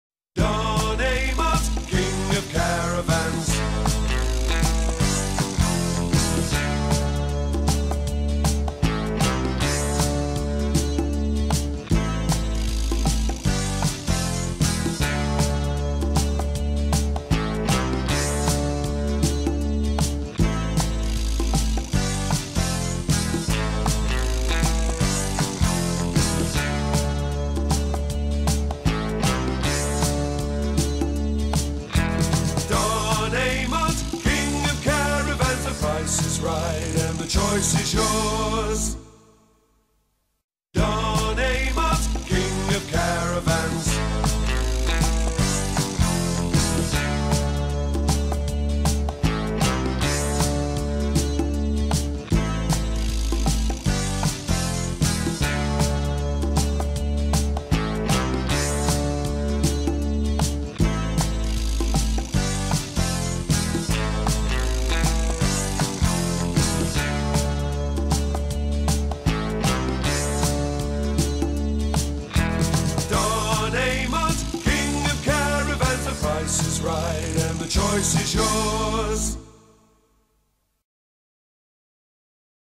Bailey Approach Autograph 745
NOW IN STOCK ! The new Bailey Approach Autograph 745 from Don Amott's Leisure Kingdom, Hilton, Derby. DE65 5FJ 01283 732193